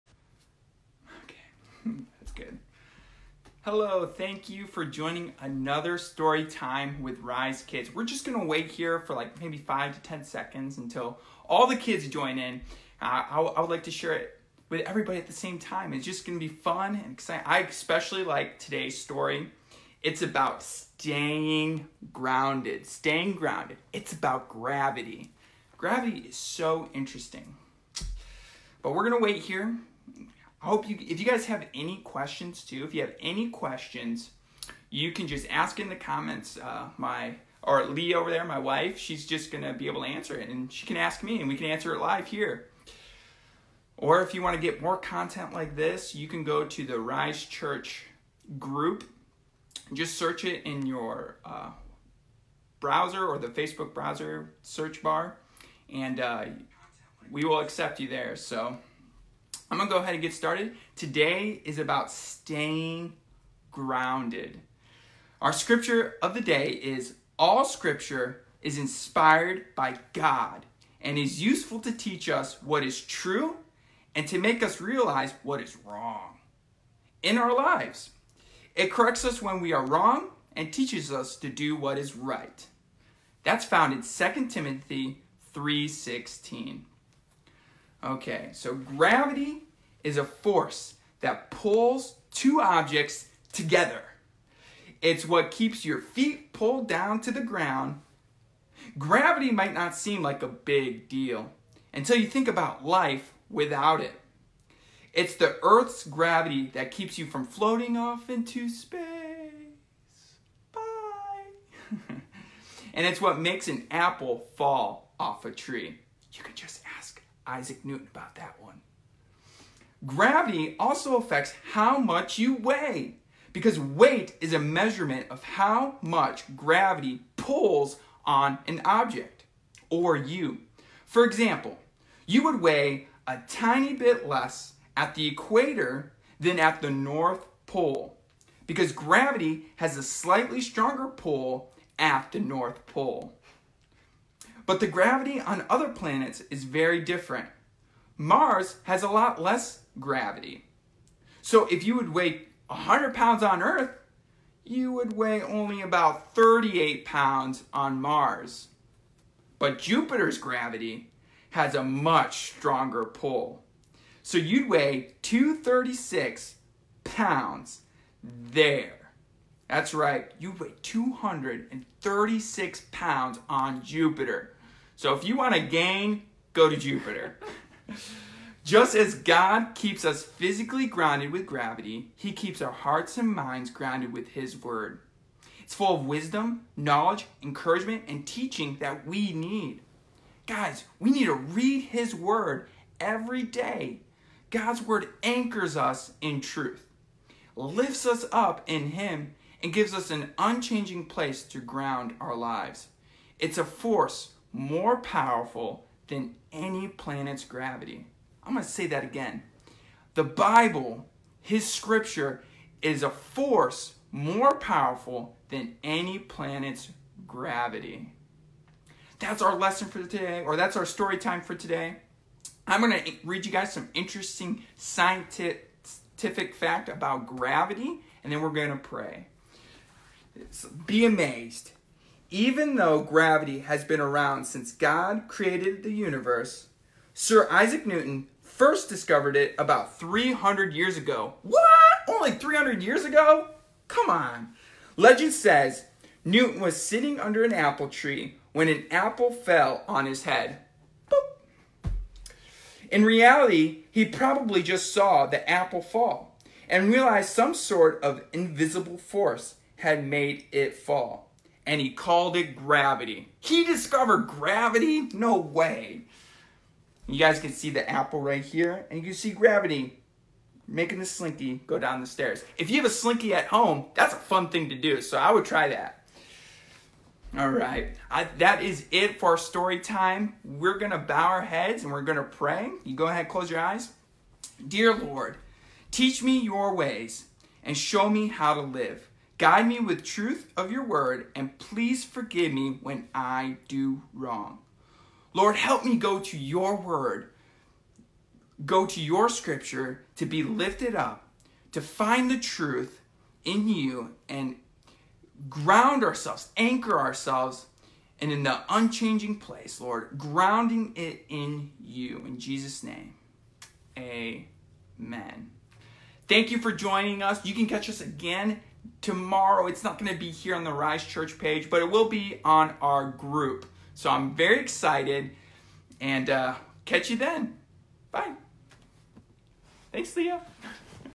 Storytime With RiseKids - Staying Grounded
Reading from Indescribable by Louie Gigilio.